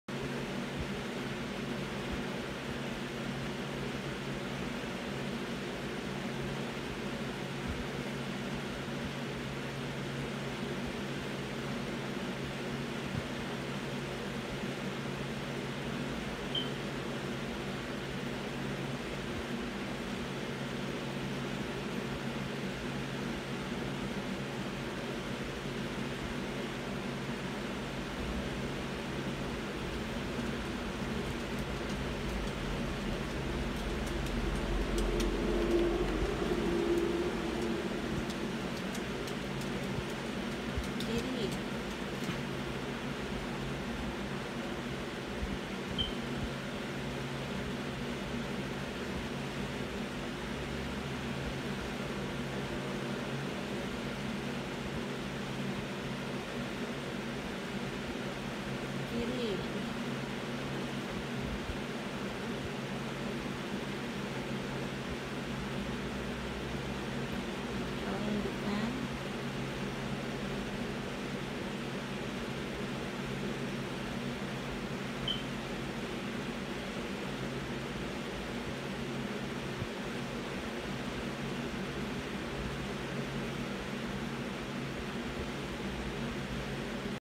Kitty Paris was feeling hot!!
Lazy hot day in front of the fan.